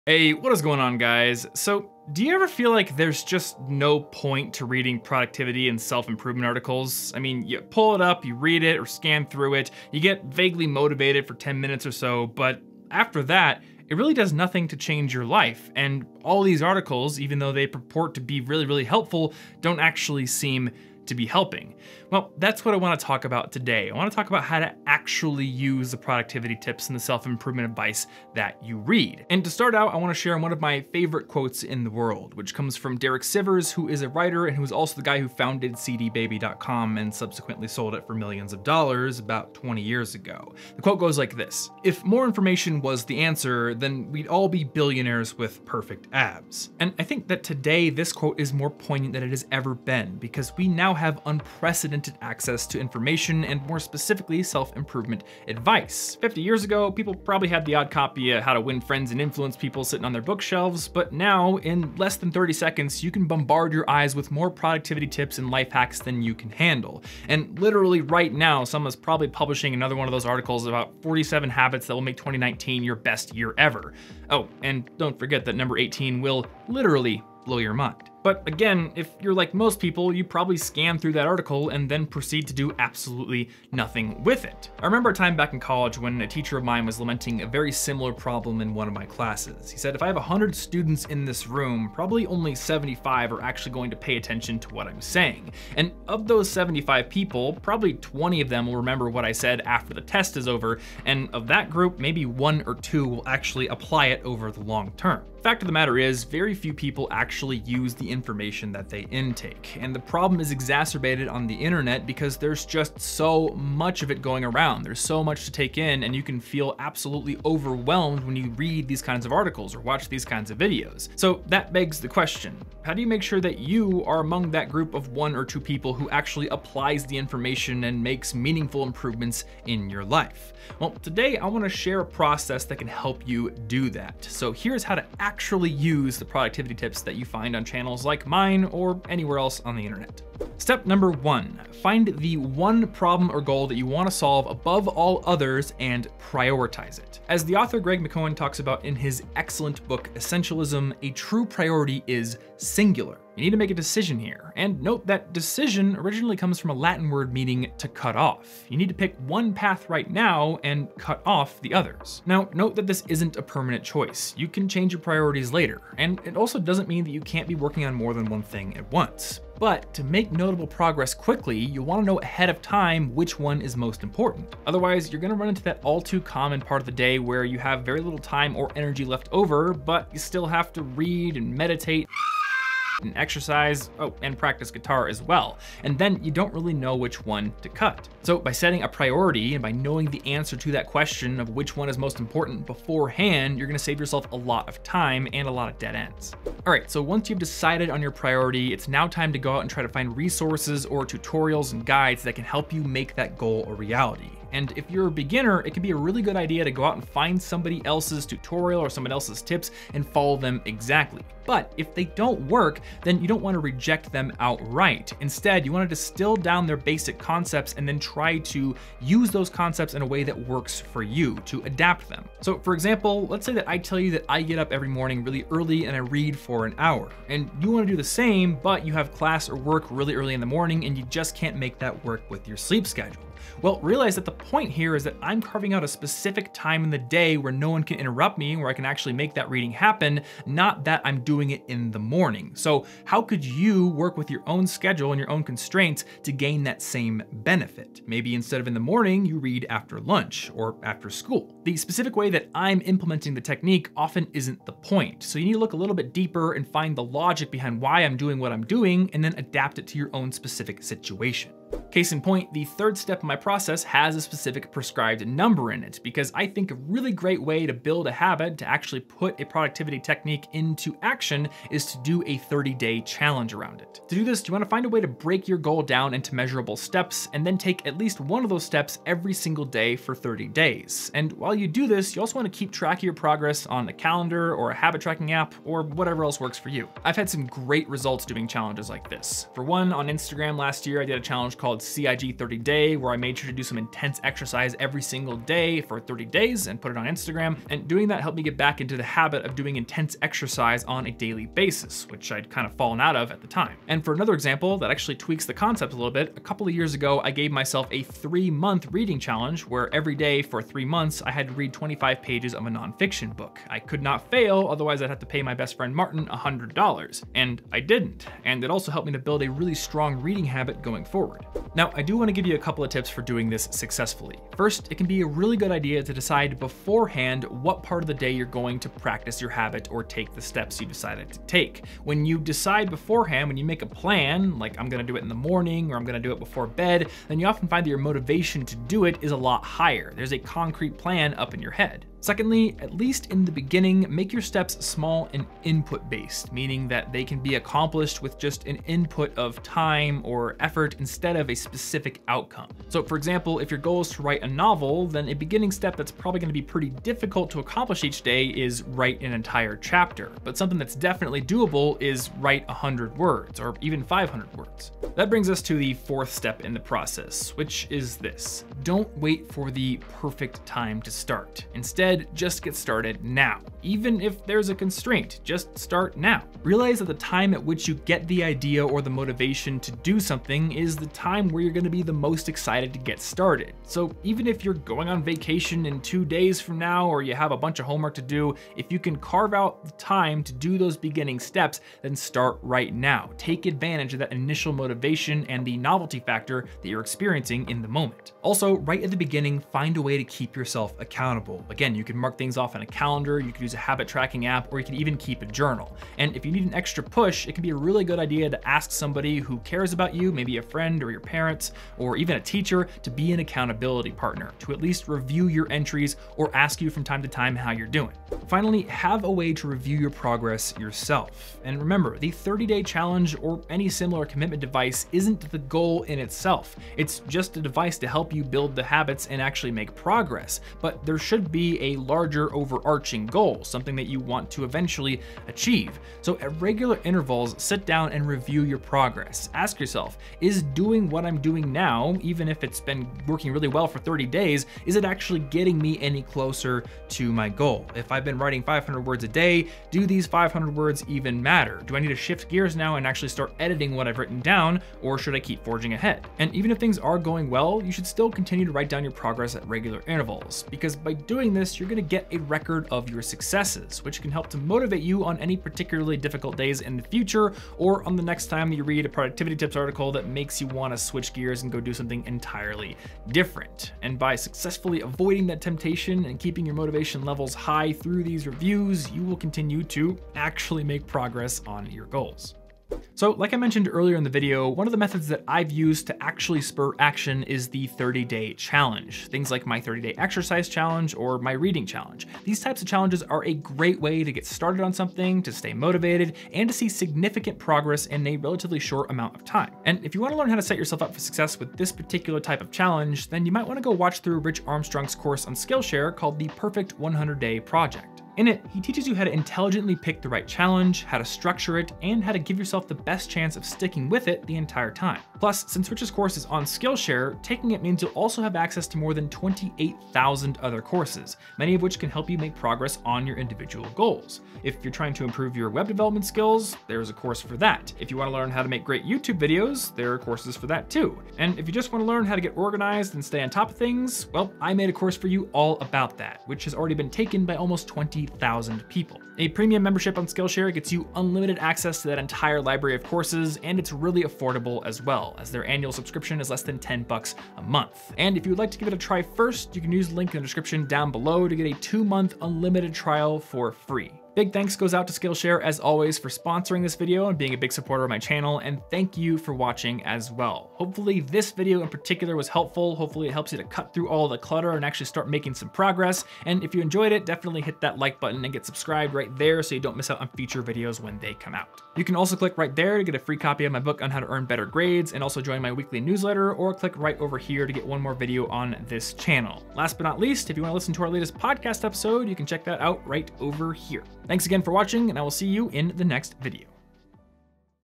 How to USE Productivity Advice (Instead of Just Consuming It)
How do you actually USE the productivity tips and advice that you're consuming? In this video, I'll share a process for doing just that.